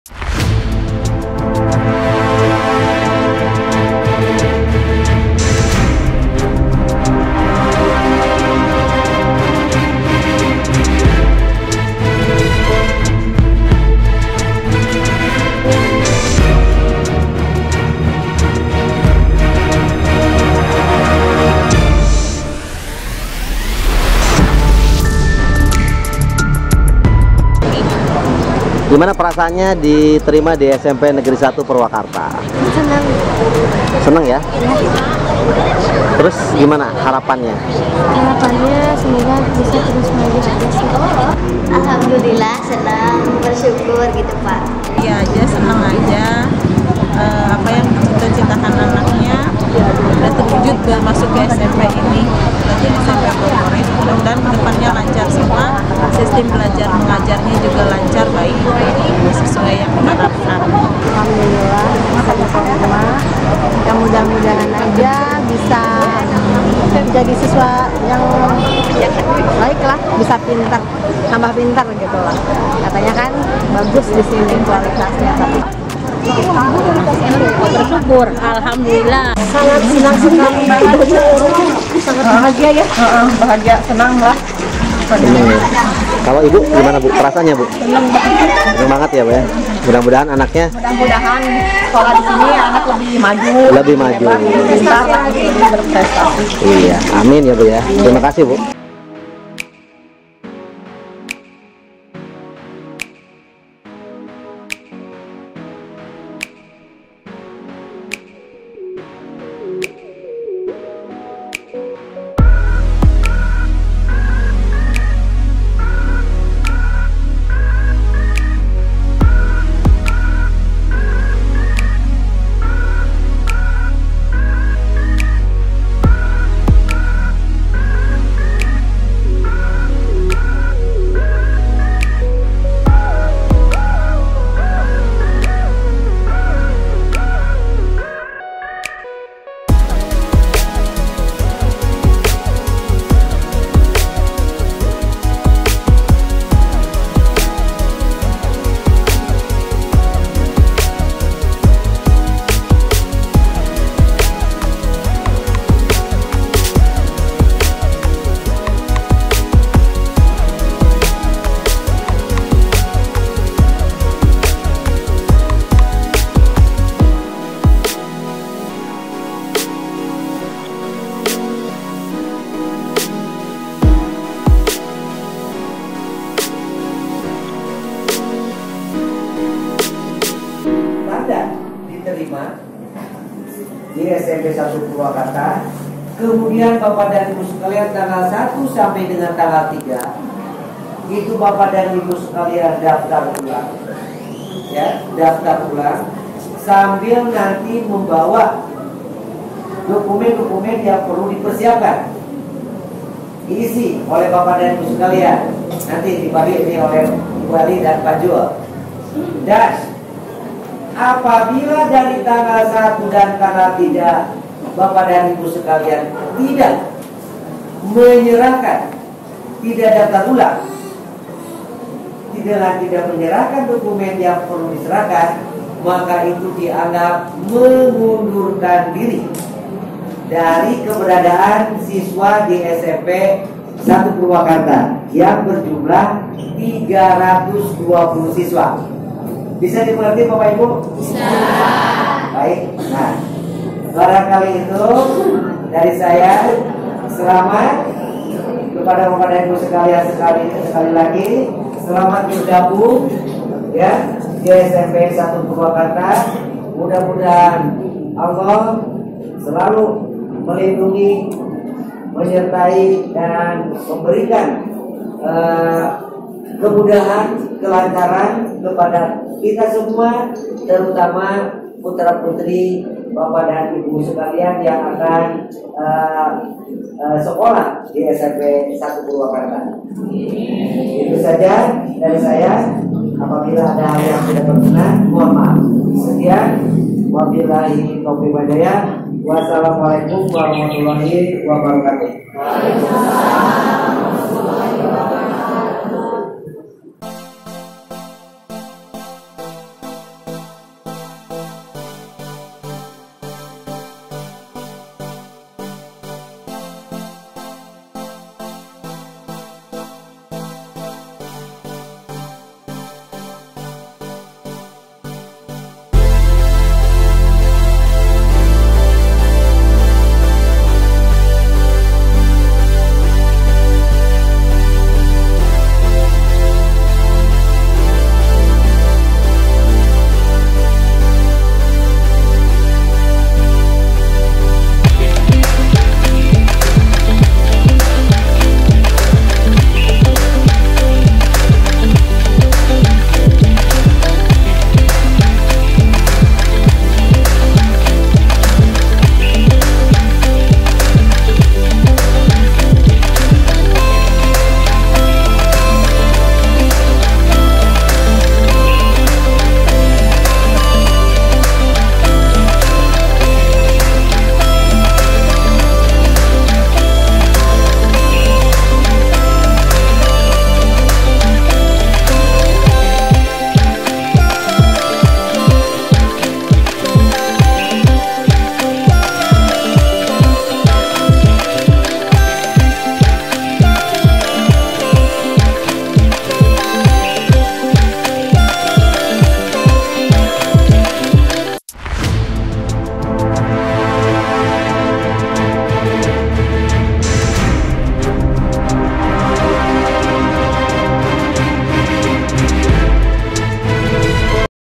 PENGUMUMAN HASIL PPDB SMP NEGERI 1 PURWAKARTA TAHUN PELAJARAN 2019/2020
Terima Kasih Kepada semua pihak Yth, untuk Bupati Purwakarta Hj. Ambu Anna Ratna Mustika, SE, Kepala Dinas Pendidikan Kabupaten Purwakarta H. Purwanto, MPd, Kepala SMPN 1 Purwakarta H . Aceng Kuswaya, M.Pd., Dan Panitia PPDB SMPN 1 Purwakarta 2019